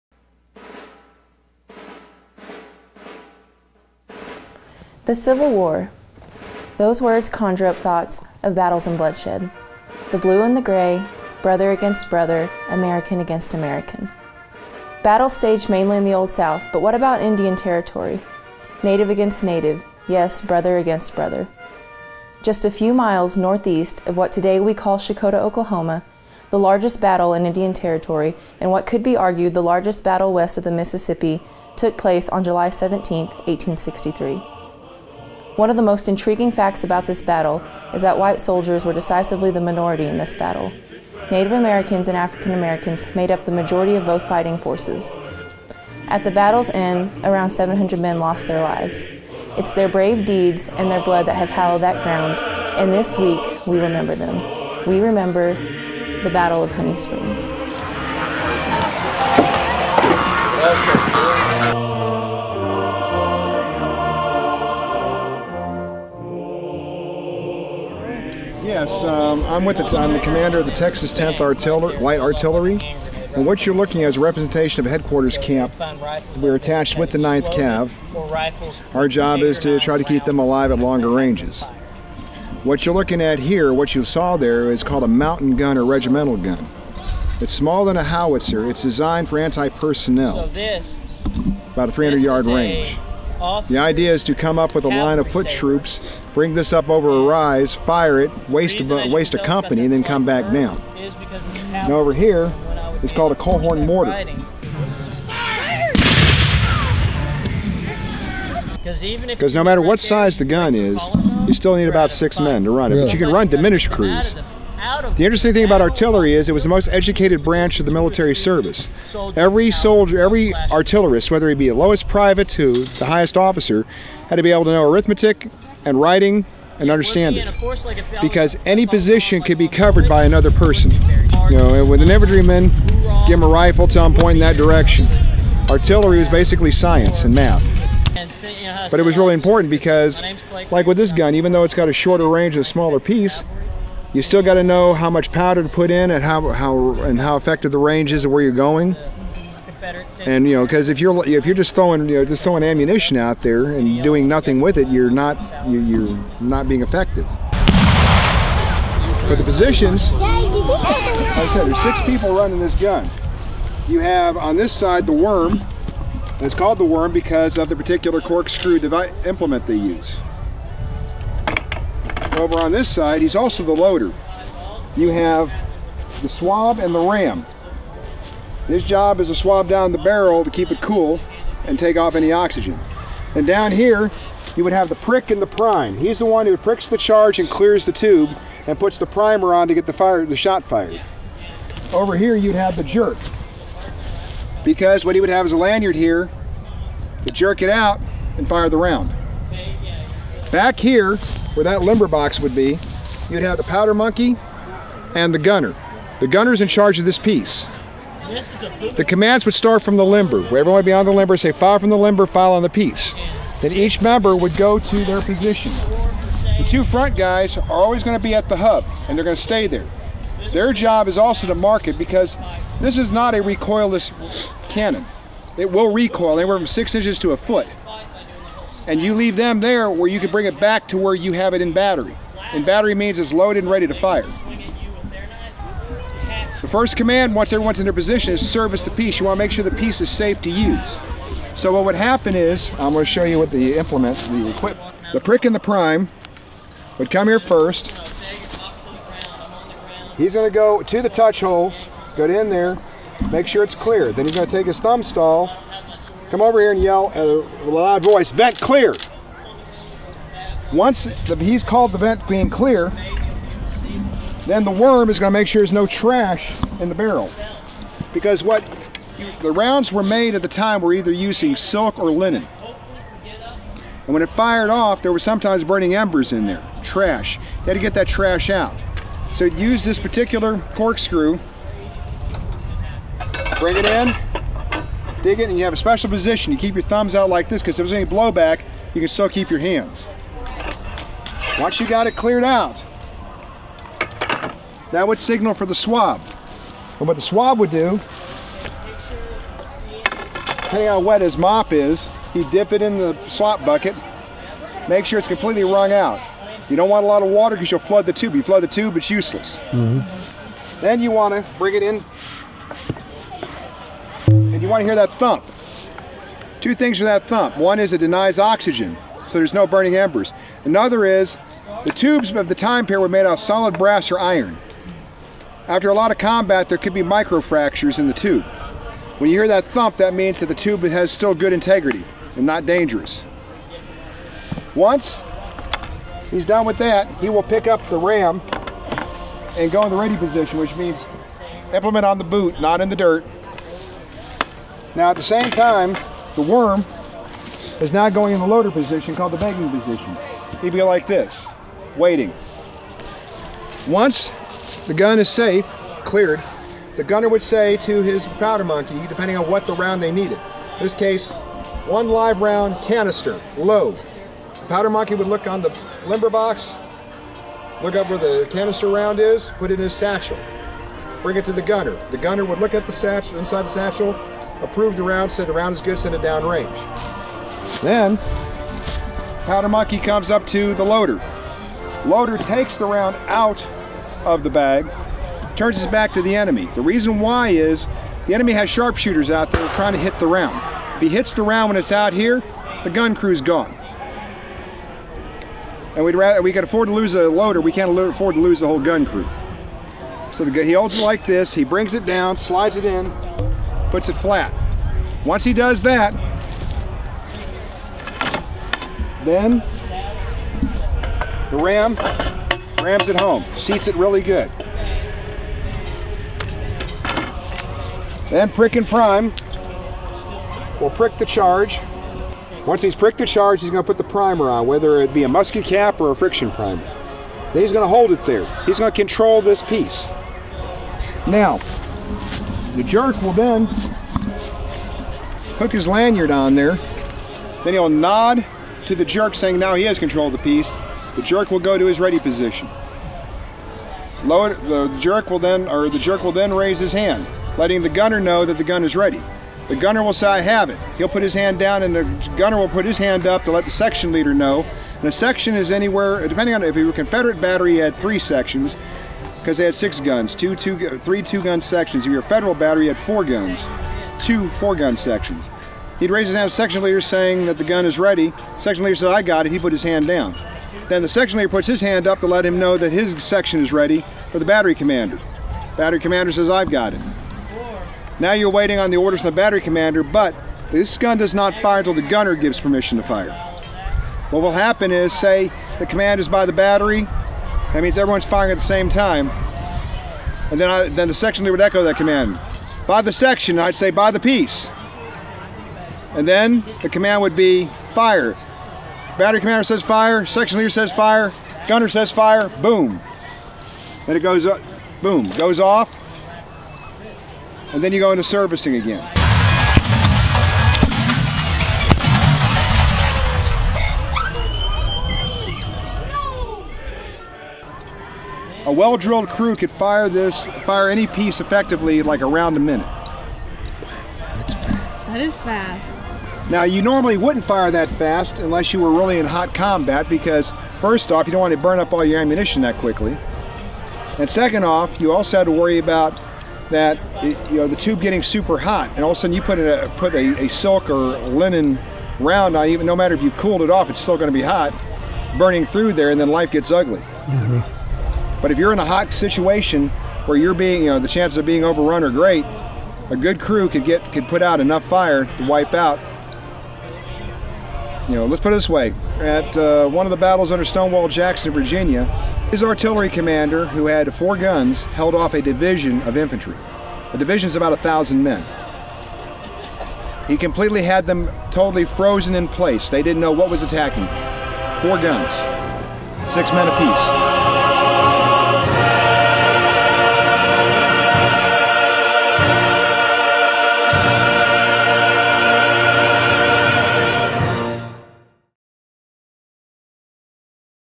Honey Springs Battlefield "the Cannon"
Rentiesville, OK— July 17, 1863. The established Confederate forces at Honey Springs had not been challenged by Federal troops for over a year. That is not until this date in 1863. The majority of the soldiers who fought in this battle were Native American with a few black soldiers fighting along side. Caucasian soldiers were the decisively the minority. The story was reenacted for public display Nov. 4, 2017
where hundreds of folks attended to witness just why the Confederate forces lost this battle. It was all in the cannons and the gun powder. Don't let your powder get wet. MVTO!